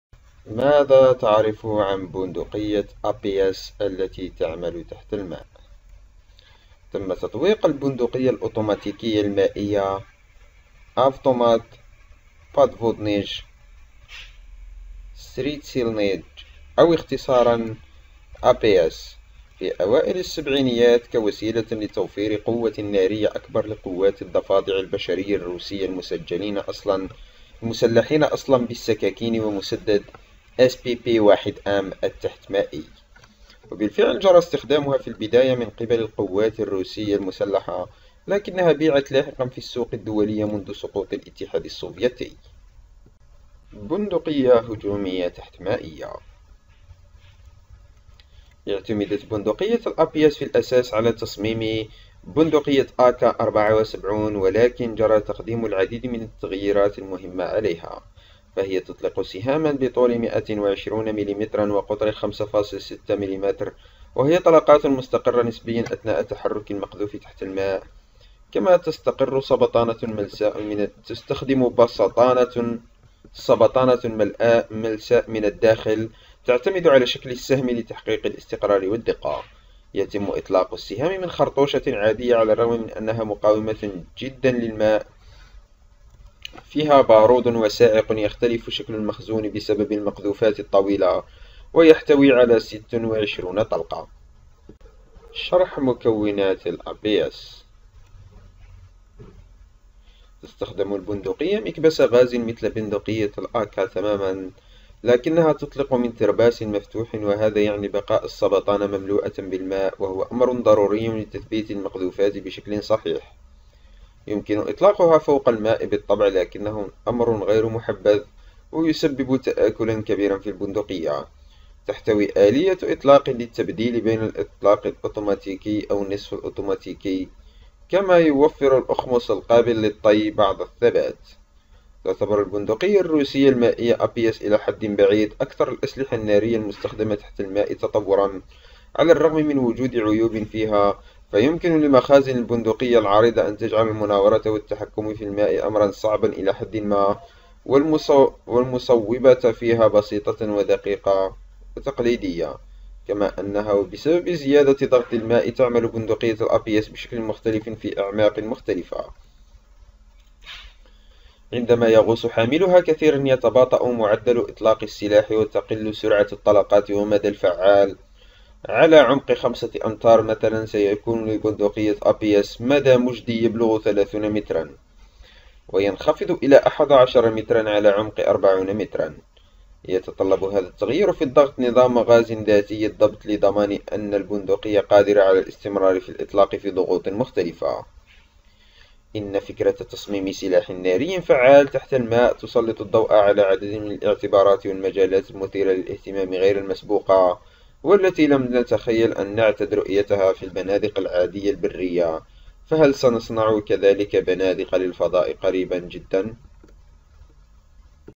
APS - تعرف على البندقية الروسية التي تعمل تحت الماء
تم تطوير ”البندقية الاوتوماتيكية المائية الخاصة“ Avtomat Podvodnyj Spetsialnyj أو اختصارا (APS) في أوائل السبعينيات, كوسيلة لتوفير قوة نارية أكبر لقوات الضفادع البشرية الروسية المسلحين أصلًا بالسكاكين ومسدس SPP-1M التحت مائي.
وبالفعل جرى استخدامها في البداية من قبل القوات المسلحة الروسية، ولكنها بيعت لاحقًا في السوق الدولية منذ سقوط الاتحاد السوفيتي.

The “Special Automatic Water Rifle” Avtomat Podvodnyj Spetsialnyj (APS) was developed in the early 1970s, as a means of providing greater firepower to Russian frog forces, originally armed with knives and the SPP-1M submarine pistol.